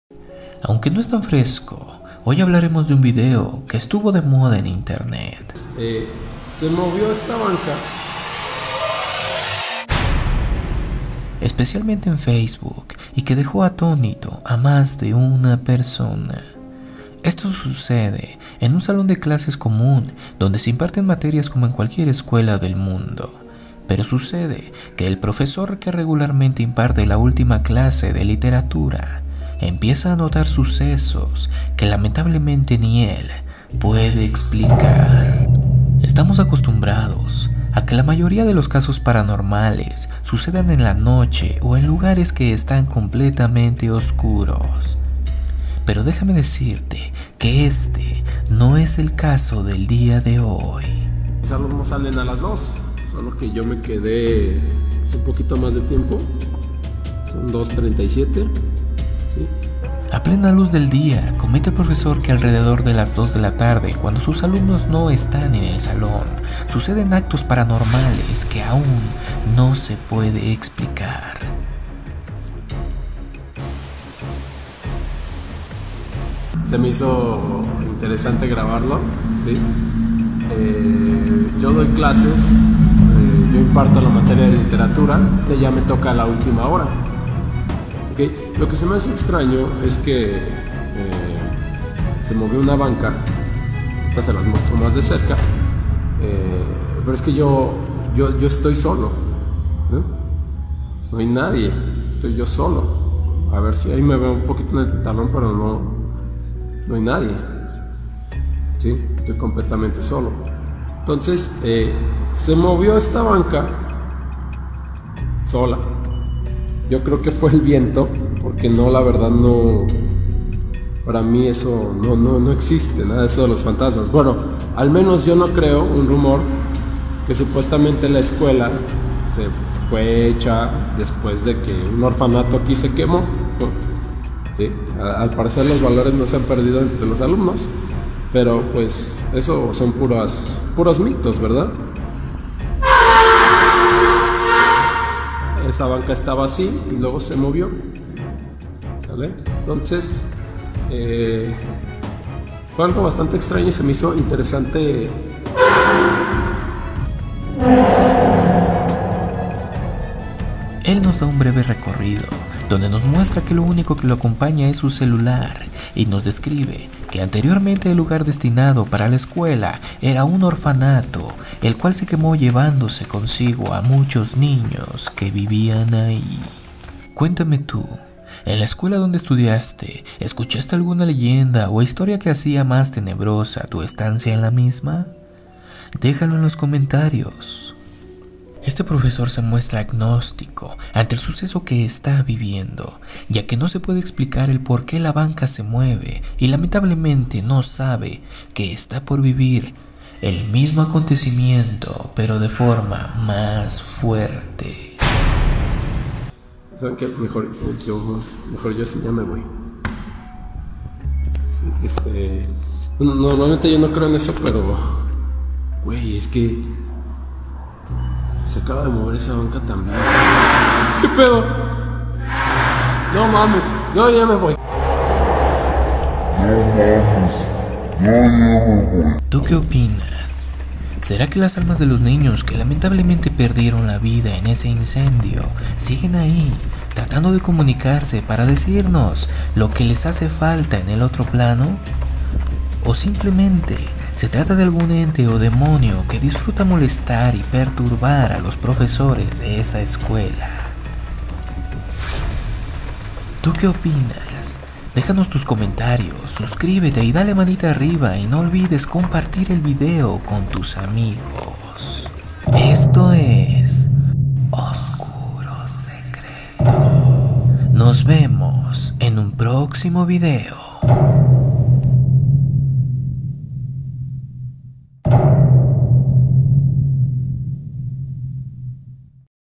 No lo vas a creer Fantasmas o demonios en salon de clases
como todos sabemos los casos paranormales están a flor de piel.
que sera?
que los espíritus quieren comunicarse con nosotros o simplemente tratan de realizar una comedia con los sustos que nos sacan al espantarnos de cierta forma.
en el vídeo de hoy te muestro la evidencia que muestra un profesor de literatura , el cual comenta que en varias ocasiones ha tenido que vivir estos acontecimientos después de clases cuando sus alumno abandonan el aula.
sera que los fantasmas a ahí habitan se sienten tristes, inconformes o molestos cuando los alumnos del salon regresan asus casas?
coméntame cual a sido tu experiencia en las escuelas.
un abrazo
saludos para toda la comunidad oscura.